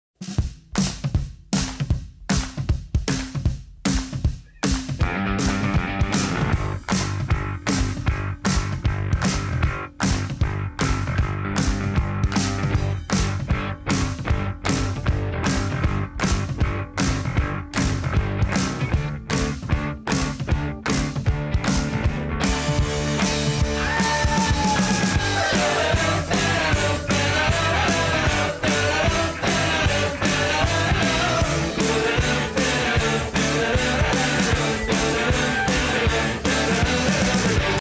How to discover Rome with friend?
Visit Rome with a Vespa with your friends